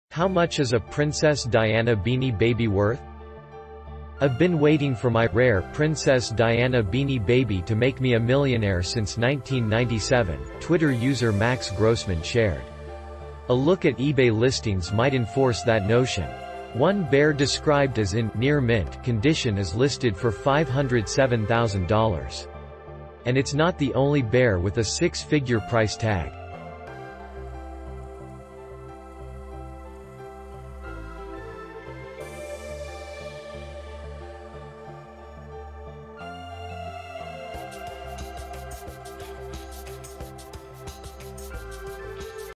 How much is a Princess Diana Beanie Baby worth?
"I’ve been waiting for my ’Rare’ Princess Diana Beanie Baby to make me a millionaire since 1997," Twitter user Max Grossman shared. A look at eBay listings might enforce that notion. One bear described as in "near mint" condition is listed for $507,000. And it’s not the only bear with a six-figure price tag.